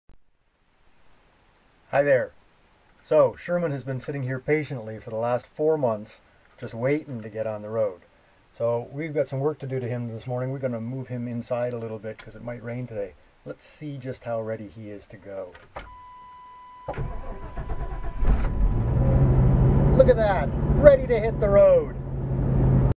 Kevin fires up the motorhome!
After sitting for 4 months, it's always a great feeling when he fires right up, raring to go!